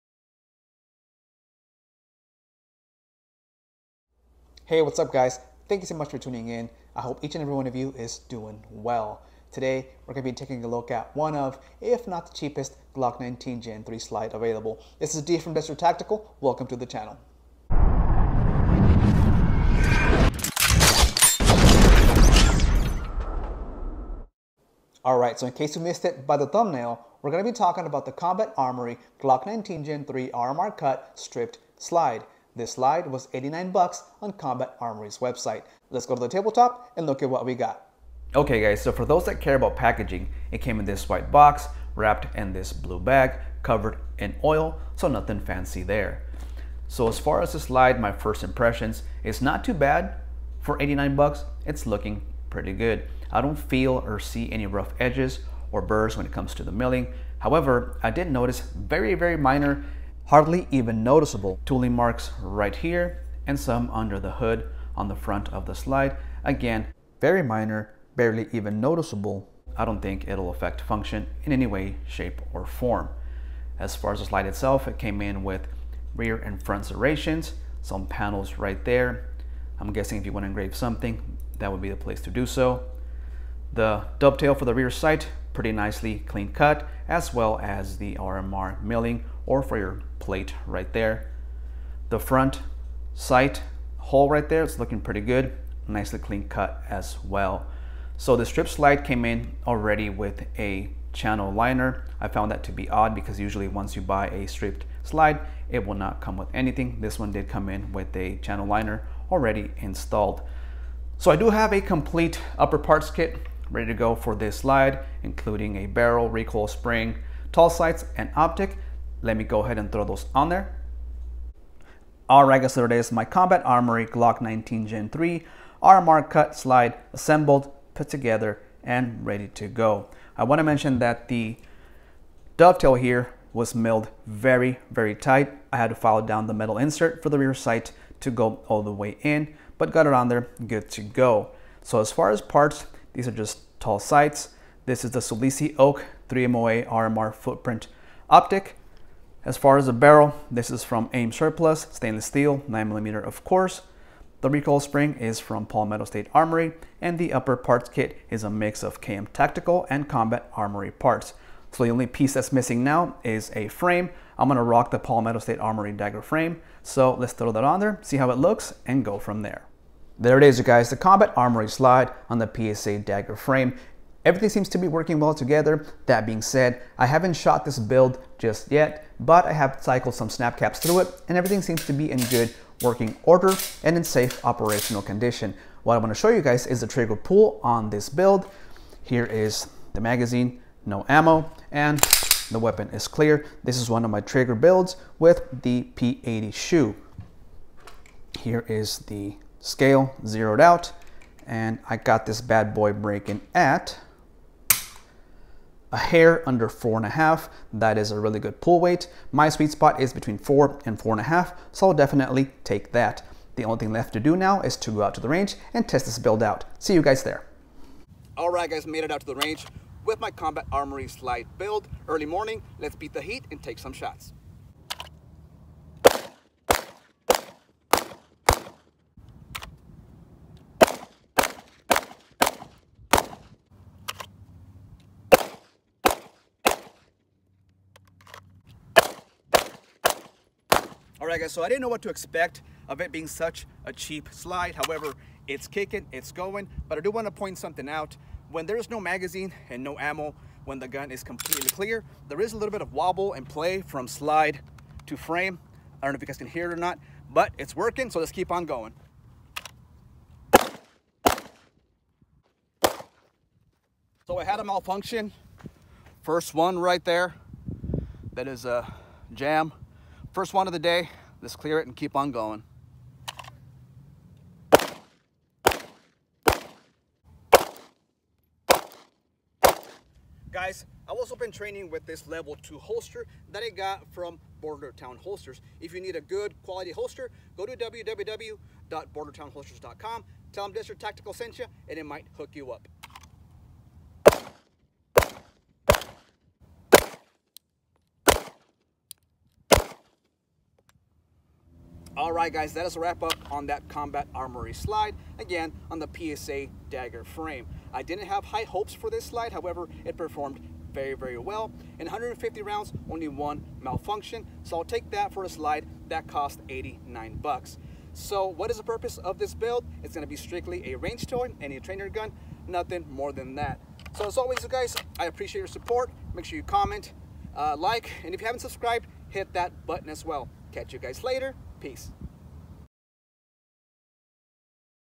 Combat Armory G19 Gen 3 Slide
Going over the Combat Armory Glock 19 Gen 3 RMR cut slide.

Its a very inexpensive slide. Not much info out there as of now so I decided to pick one up and test it out.

Also testing out a level 2 Holster from Bordertown Holsters. Huge thanks to them since they are a sponsor of the channel.

* NO SALES OF ANY KIND *

Please like, drop a comment and sub!